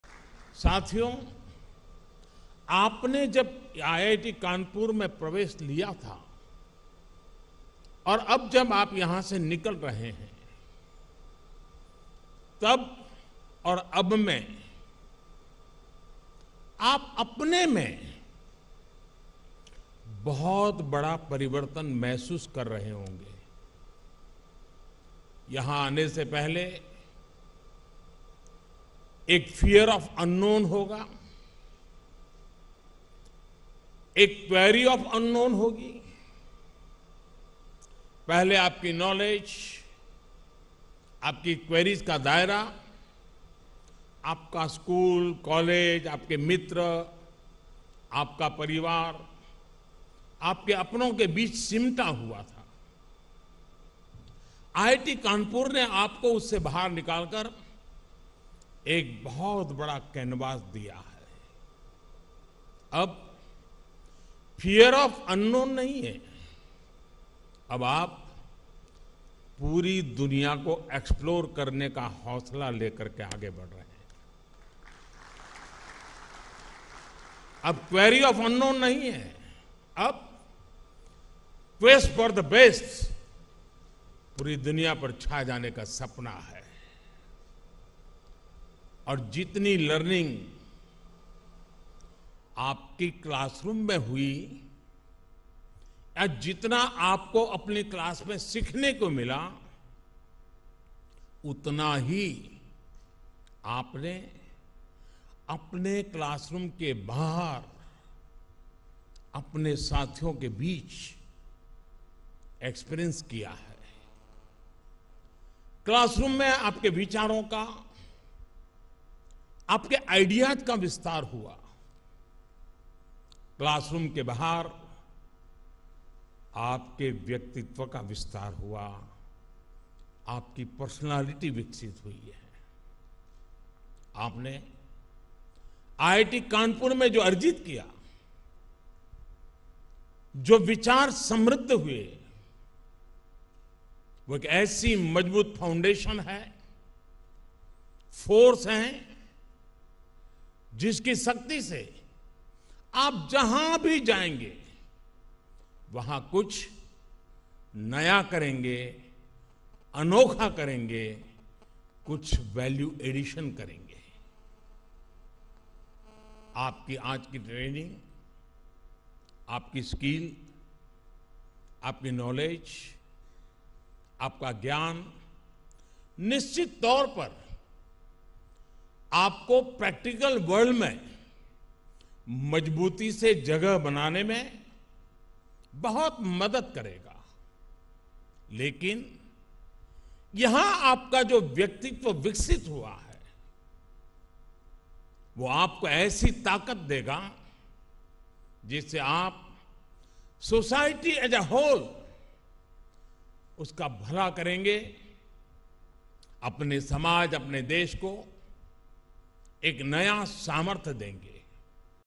IIT Kanpur has provided the platform to step out and explore the world: PM Modi
Prime Minister Narendra Modi attended the 54th Convocation Ceremony of IIT Kanpur. The PM urged the students to become impatient for a self-reliant India. He said, "Self-reliant India is the basic form of complete freedom, where we will not depend on anyone."

pm modi in kanpur
pm modi in iit kanpur
pm modi address convocation Ceremony
latest uttar pradesh news